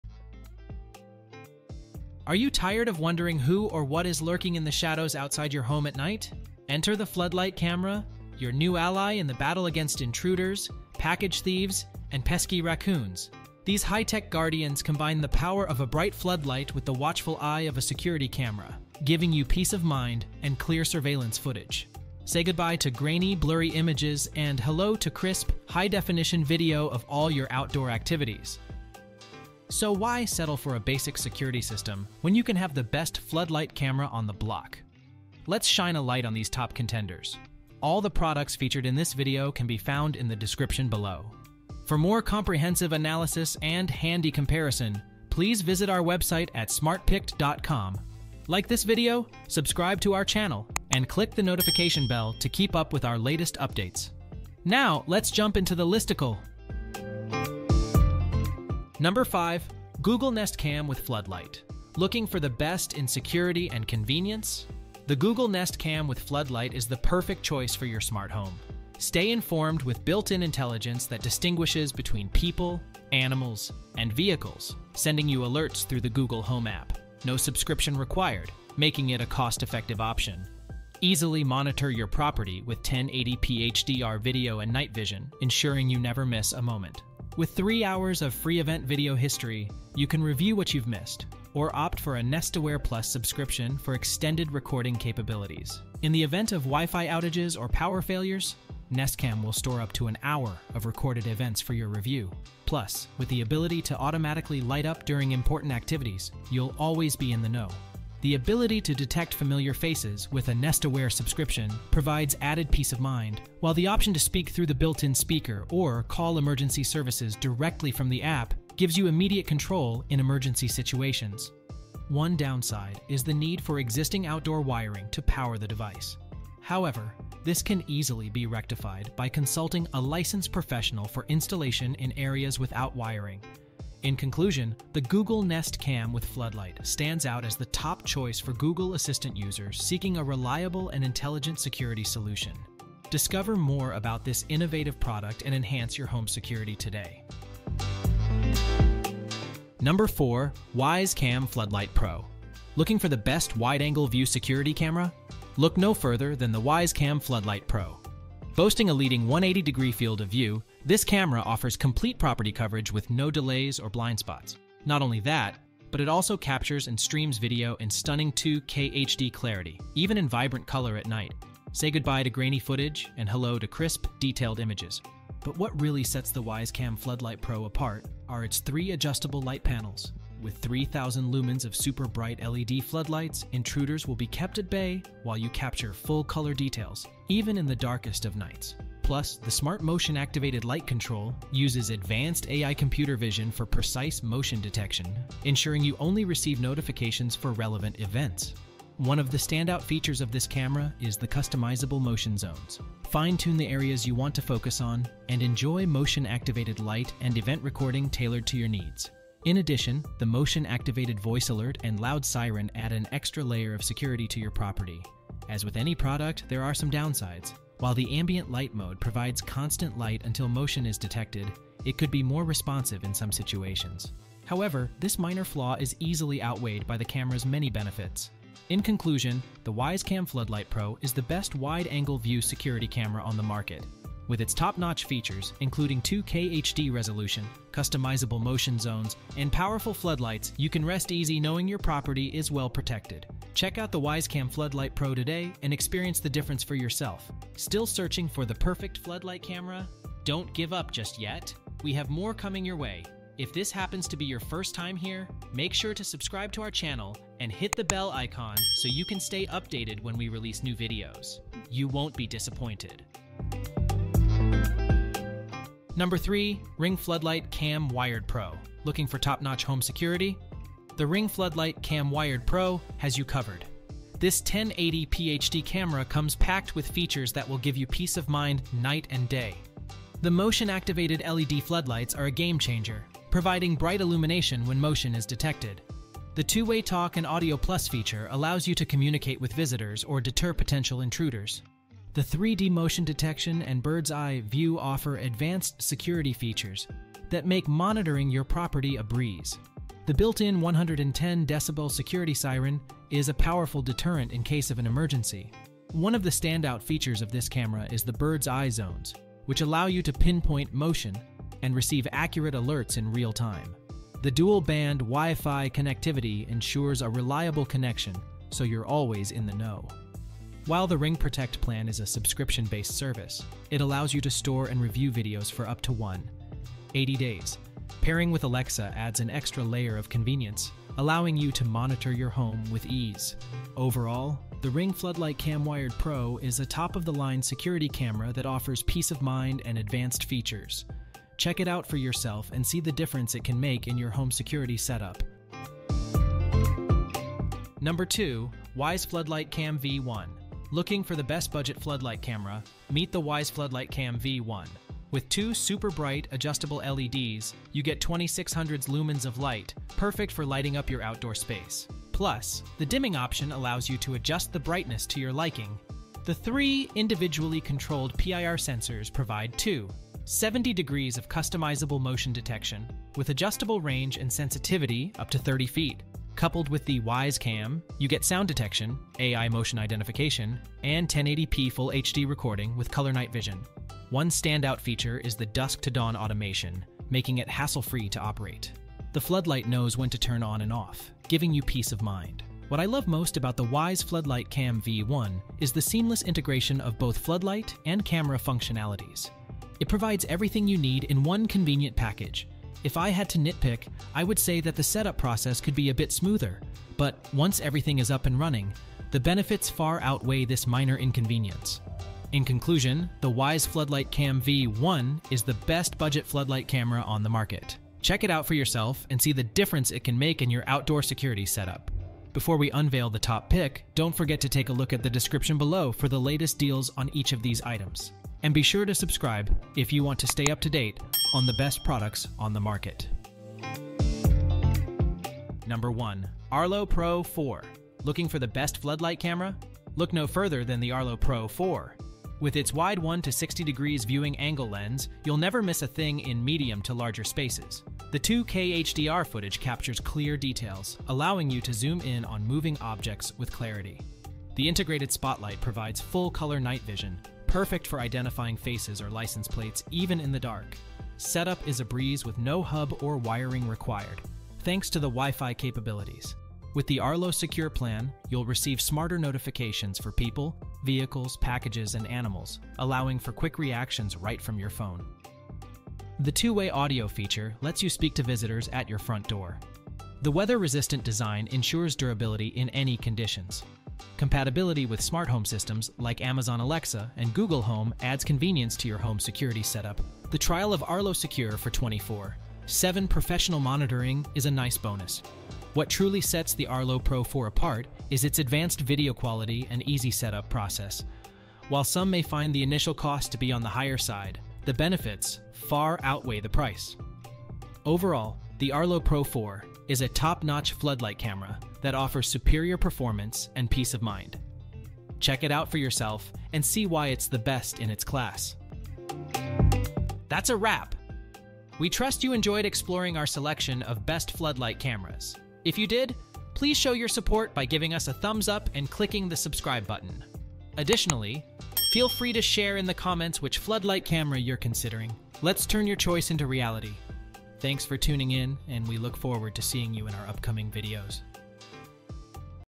Best Floodlight Cameras 2024 | Which Floodlight Camera Should You Buy in 2024?
Illuminate your security game with the Best Floodlight Cameras of 2024, providing advanced features for enhanced protection day and night!

➡ Links of the Best Floodlight Cameras we listed in this video.

👉Best Floodlight Camera Overall
1. Arlo Pro 4

👉Best Budget Floodlight Camera
2. Wyze Floodlight Cam v1

👉Best Detection and Tracking Features
3. Ring Floodlight Cam Wired Pro

👉Best Wide-Angle View
4. Wyze Cam Floodlight Pro

👉Best for Google Assistant
5. Google Nest Cam with Floodlight

00:00 Intro
00:57 Google Nest Cam with Floodlight
02:27 Wyze Cam Floodlight Pro
04:23 Ring Floodlight Cam Wired Pro
05:45 Wyze Floodlight Cam v1
07:13 Arlo Pro 4
08:38 Outro

💡📹 Taking the lead is the Arlo Pro 4, our top pick for the Best Floodlight Camera Overall, offering crisp video quality and versatile features. For those on a budget, the Wyze Floodlight Cam v1 shines as the Best Budget option, providing reliable surveillance without breaking the bank. Stay ahead of potential threats with the Ring Floodlight Cam Wired Pro, recognized for its Best Detection and Tracking Features, ensuring you never miss a moment. Enjoy wide-angle views with the Wyze Cam Floodlight Pro, our choice for the Best Wide-Angle View, capturing more of your surroundings for comprehensive monitoring. Seamlessly integrate with your smart home ecosystem with the Google Nest Cam with Floodlight, crowned as the Best for Google Assistant, offering seamless integration and voice control. Choose the floodlight camera that fits your security needs and enjoy peace of mind knowing your property is protected. 🔒🏡